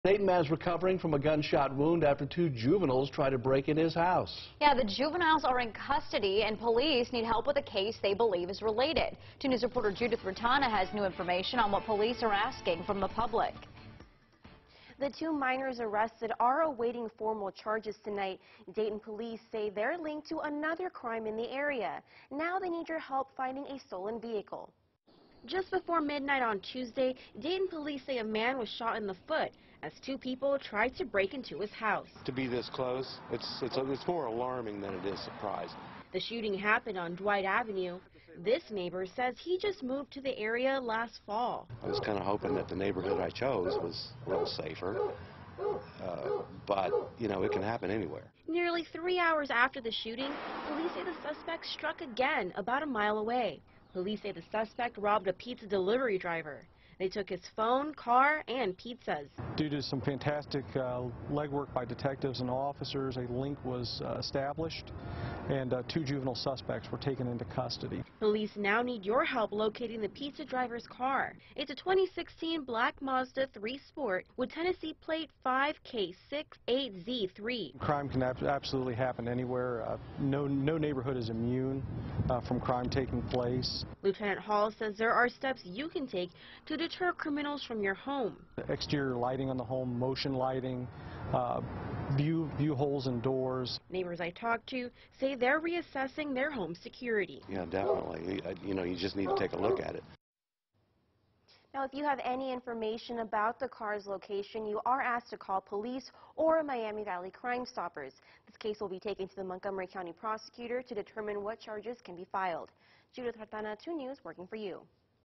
2 arrested following home invasion, stolen car still missing
2 arrested following home invasion, stolen car still missing